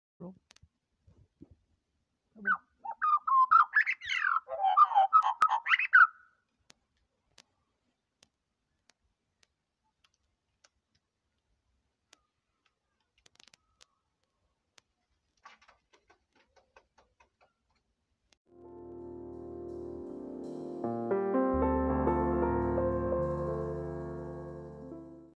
Singing Maggie Gets a Feed
See Magpie sing for its supper... watch it be hand fed.
Filmed in Western Australia 2022.